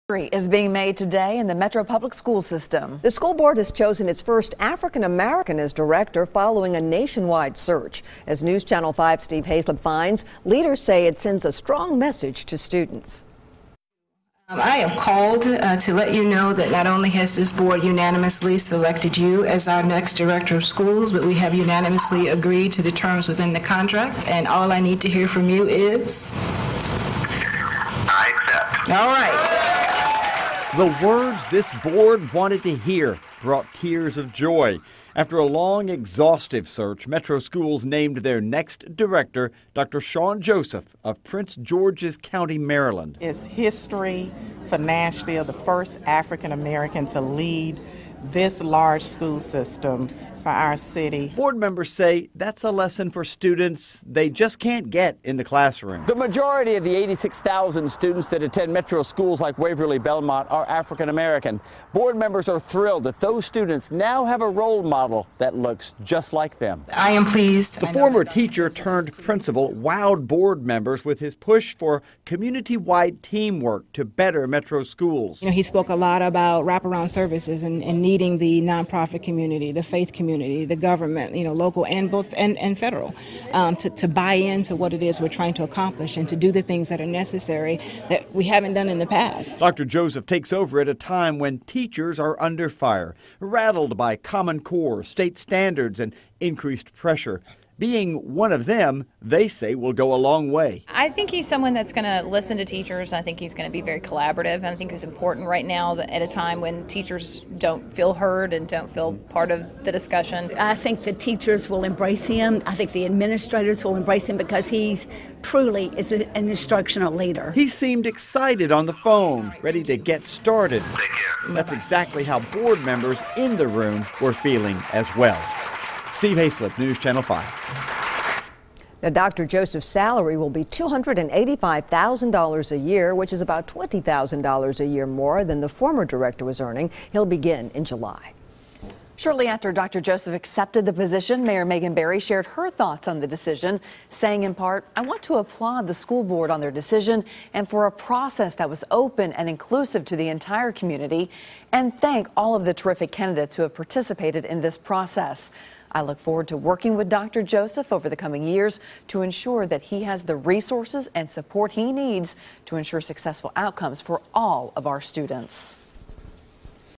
Shawn Joseph Accepts MNPS Director Position
Dr. Shawn Joseph has accepted an offer to become the next Director of Metro Nashville Public Schools.